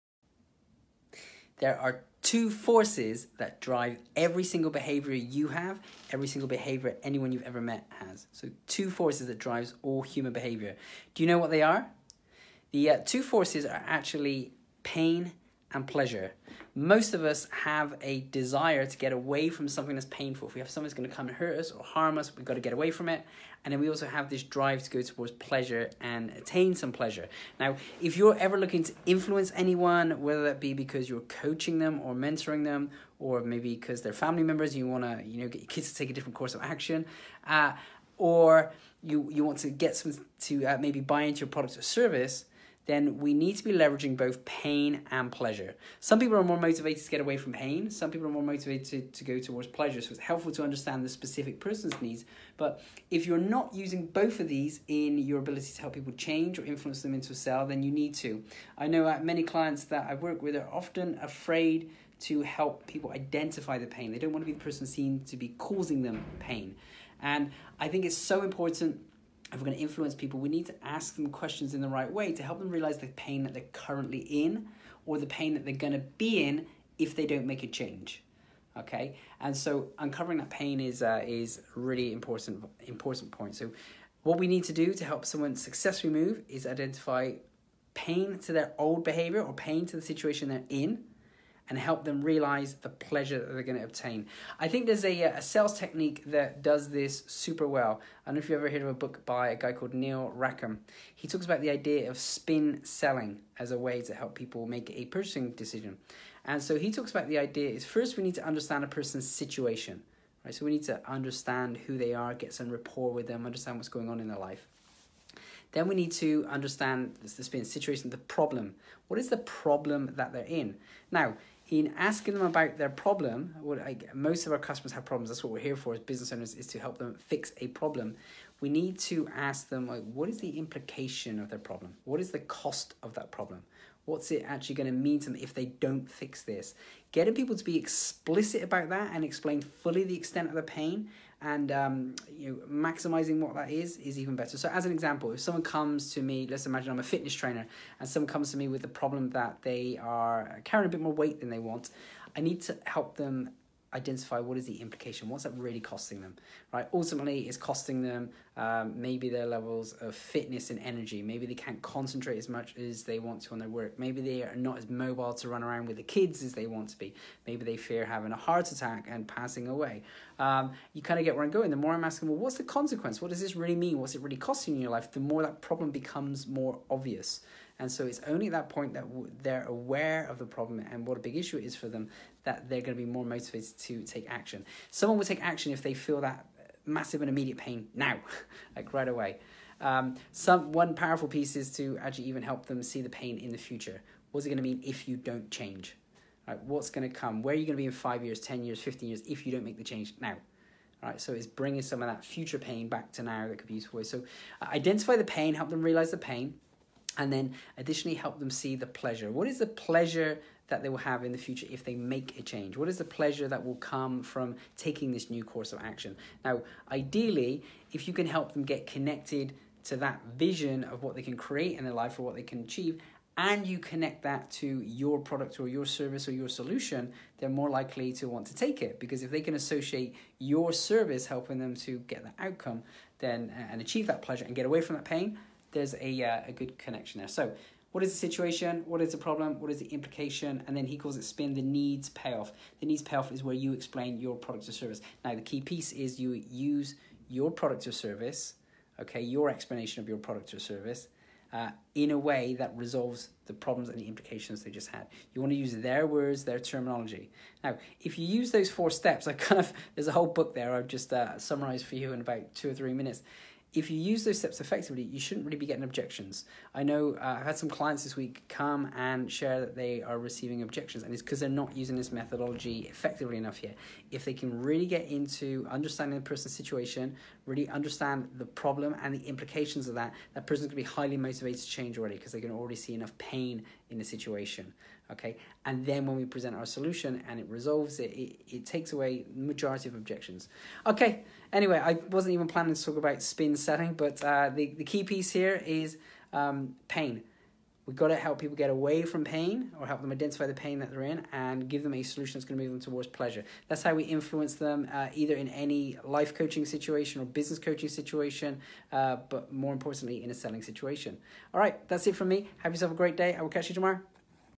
Twin Forces That Drive All Behaviour - Life Changing Business #47
Business Growth Expert Kevin Bees is sharing Life Changing Business Tips.

Today,  There are two forces that drive every behavior in life.

Pain & Pleasure

We can be driven to get away from pain
Or to go towards pleasure

You need to leverage both if you are to convince someone to take anew course of action!

Pain to the current situation
Pleasure to the new.

How well do you know:
The pain your prospects are in?
What are their biggest frustrations?

The pleasure your prospects want?
What do they most desire?

Take some extra time this week and really listen to what they’re telling you, and note down the words they use, this will become massively important to use in your ongoing marketing and sales.

Example of how to use SPIN Selling Methodology